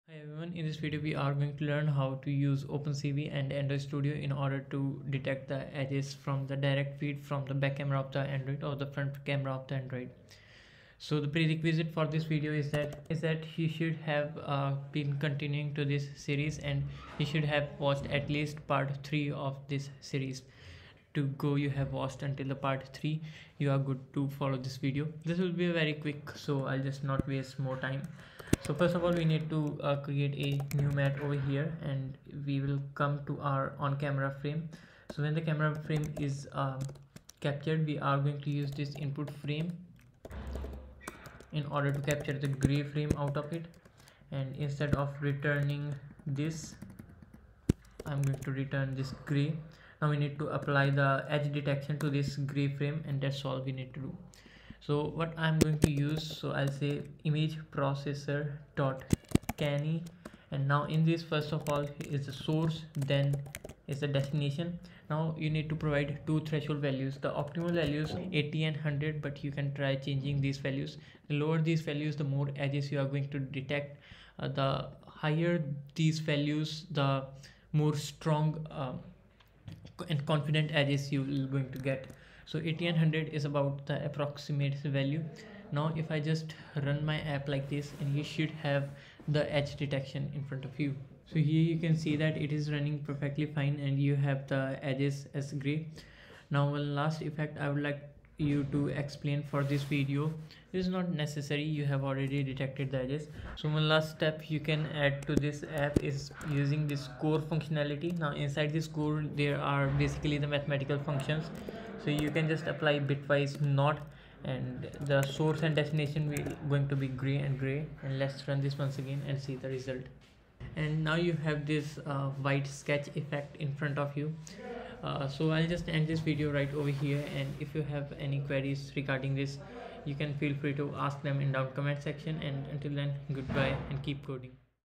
#6 | edge detection | java | image processing android | opencv android studio
Hi in this video which is quick one but you will learn about canny edge detection and using it in the android using java language. Opencv module is used for the edge detection in android.

This is further proceeding to the playlist of opencv and I hope you will enjoy this video too.
Thanks :)

opencv,android,studio,java,image processing,imgproc,core,bitwise_not,opencv android,how to,how